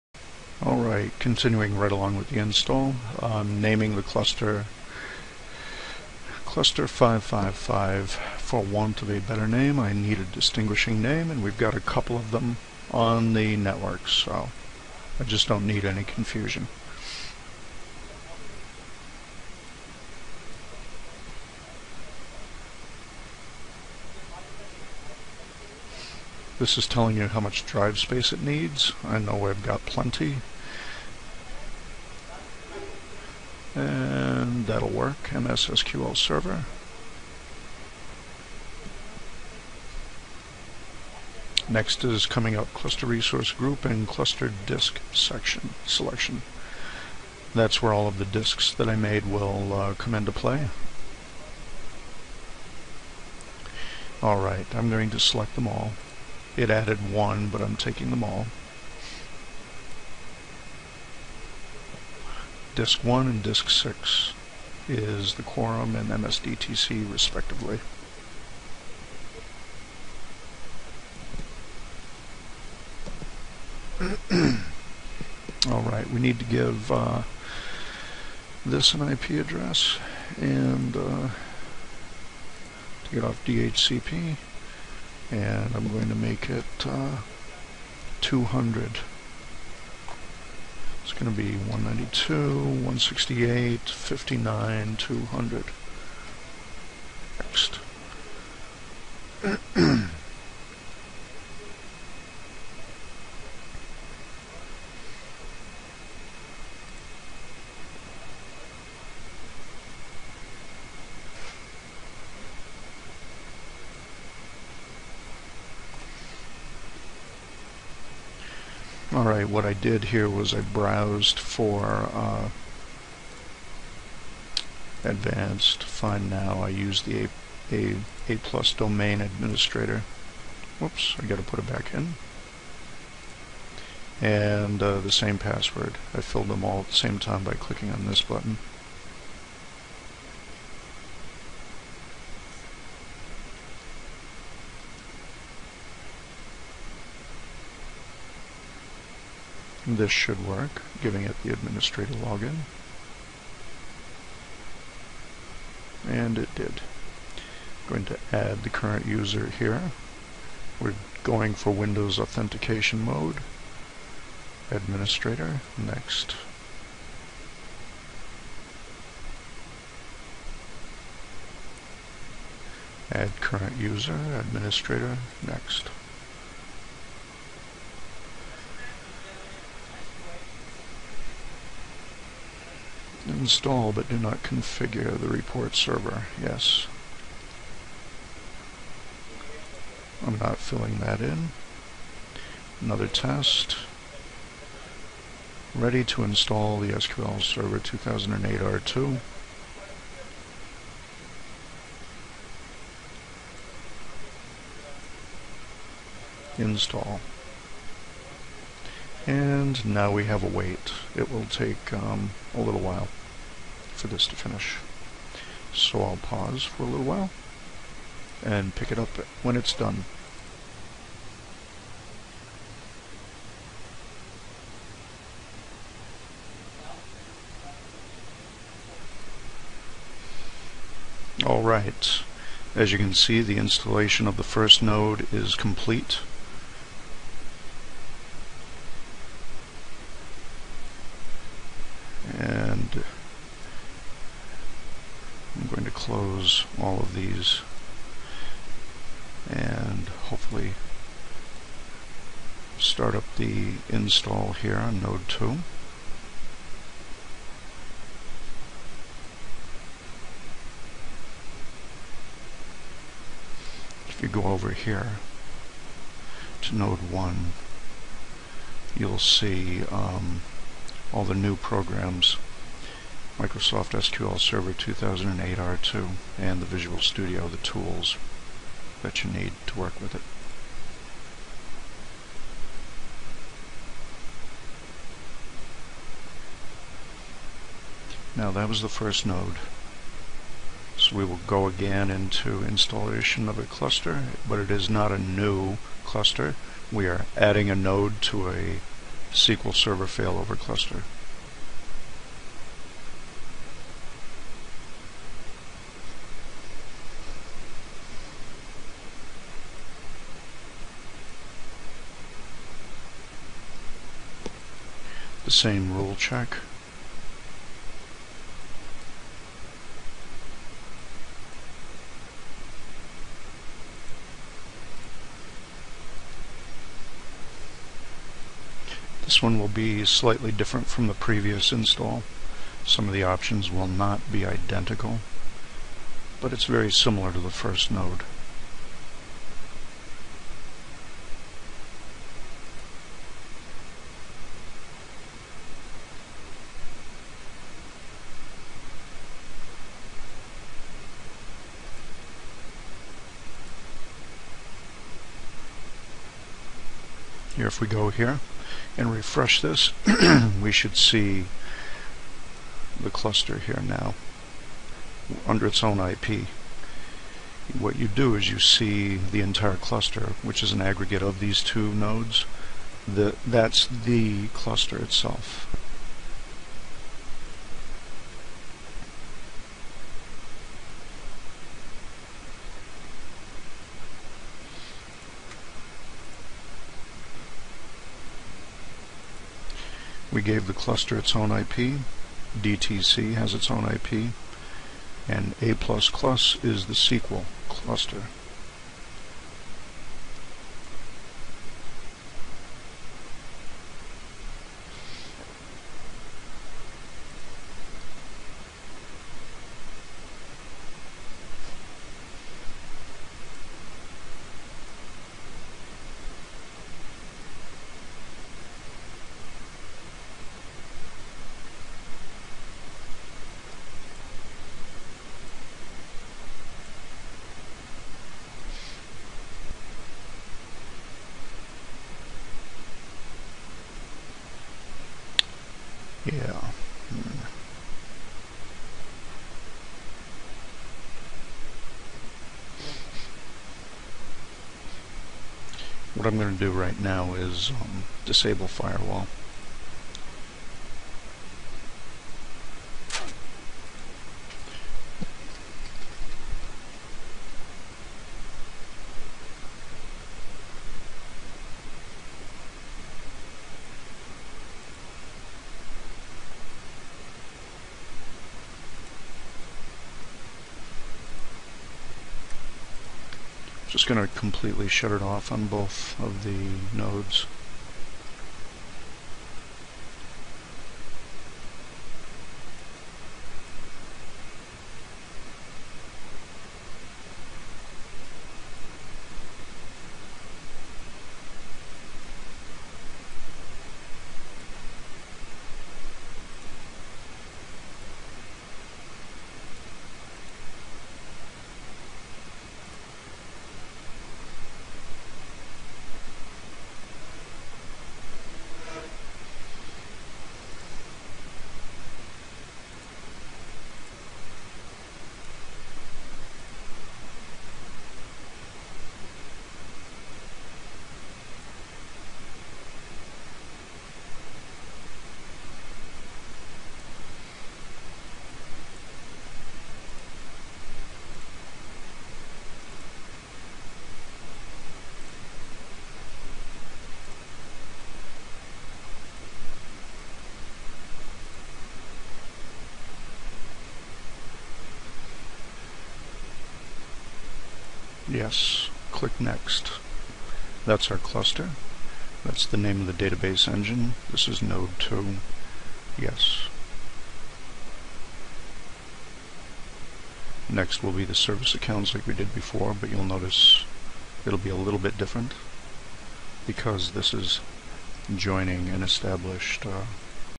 2 Node SQL HA Cluster under vmWare Workstation - 07
Part 7 of 8, SQL HA Cluster series by StvRich of AutomationDnD.  This type of material will be IN a new course I am making that is Data Center Infrastructure training.  This new course will (ultimately) see you setup a cluster in vmWare,  (with testing on it)   and also Cisco infrastructure.  But the "course" itself is a VCA-VDC prep-style course at the VCA level.   the "Good Stuff" is the Labbing / infrastructure exposure... this is all SDDC orientation and training.